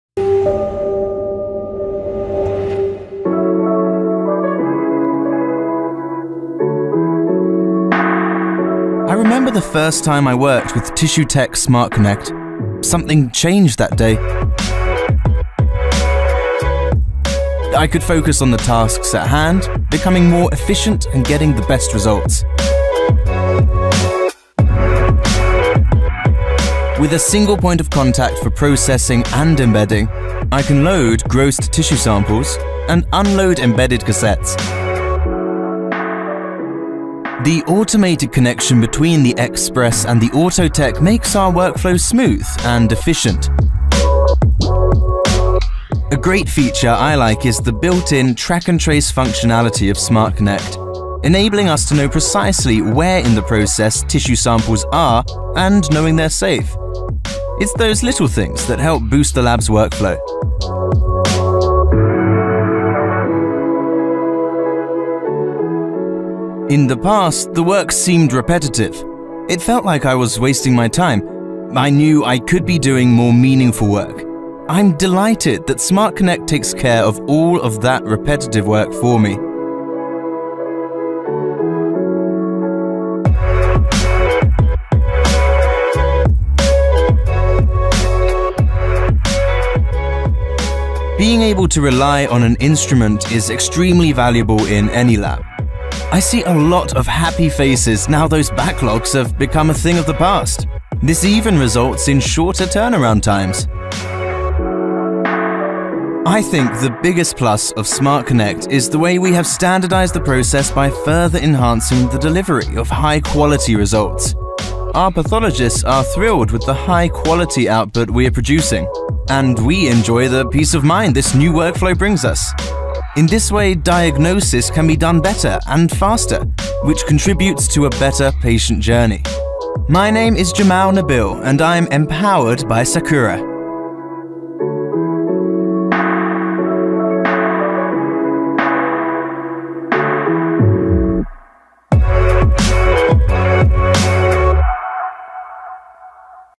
Tissue Tek SmartConnect® | Product video
The Tissue-Tek SmartConnect® is a robotic tissue transferring solution from Sakura, automating workflow in the lab between processing and embedding. Save valuable time and focus on what matters most. Discover more

EMVID0071EN Rev. 01